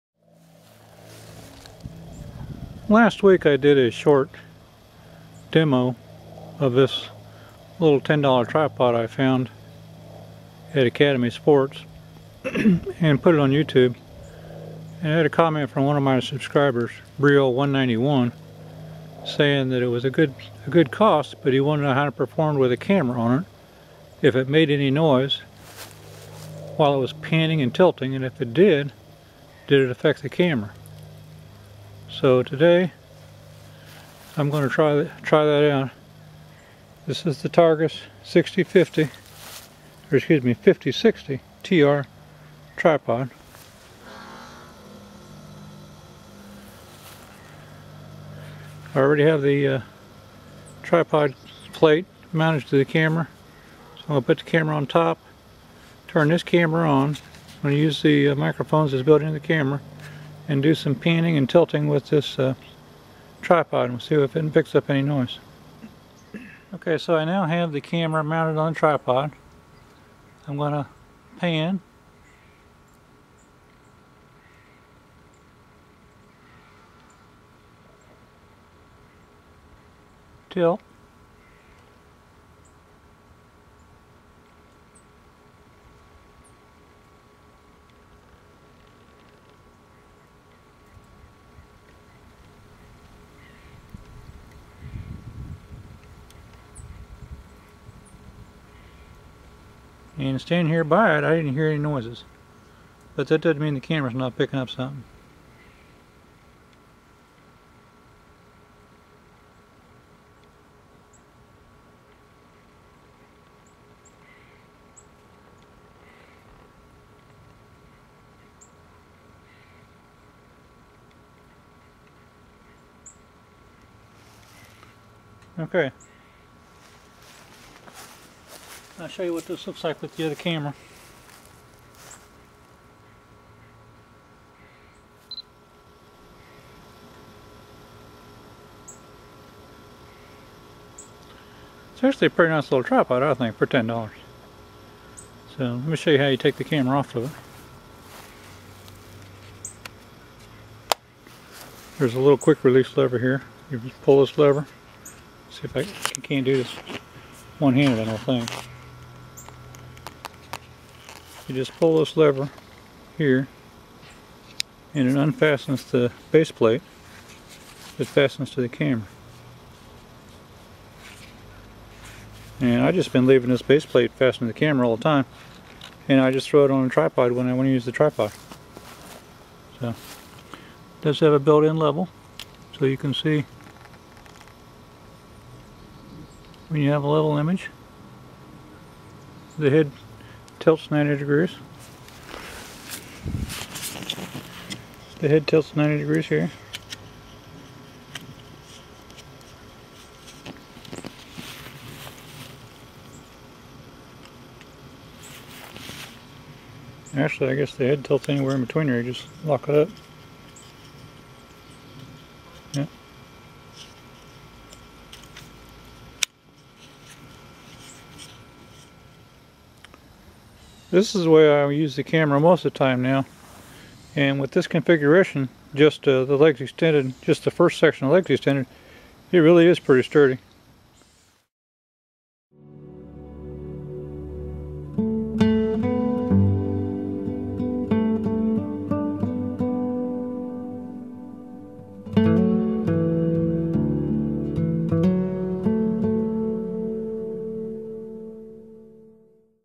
lightweight Targus TG5060TR tripod -- pan/tilt test. wmv
The tripod is rated for 2.5 pounds (DSLR?) but I would only expect that capacity with the legs retracted. When the legs are fully extended, I would limit it's use to the typical point & shoot still cameras and compact camcorders. Length: 16.5", weight 18 oz.